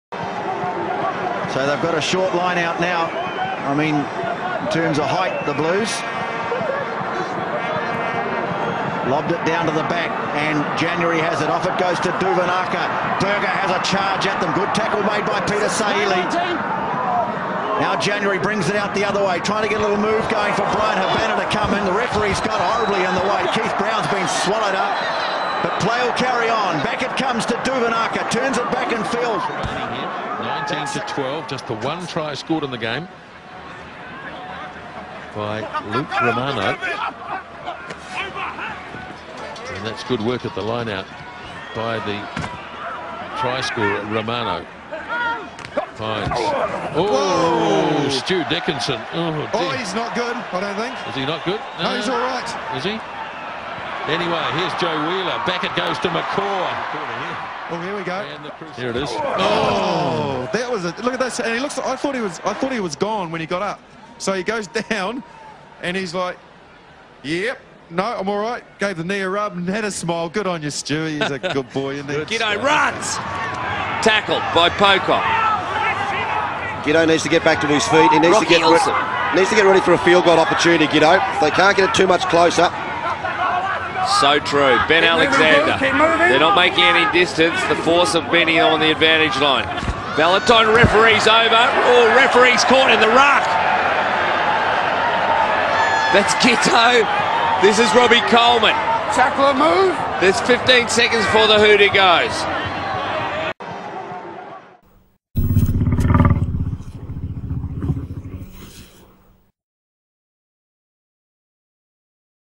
3 arbitres sur le cul, dans le même week end
Keith Brown, Stuart Dickinson, et Ian Smith sont chargés par des joueurs, lors du même week-end.
Rude est le rugby, même pour les arbitres.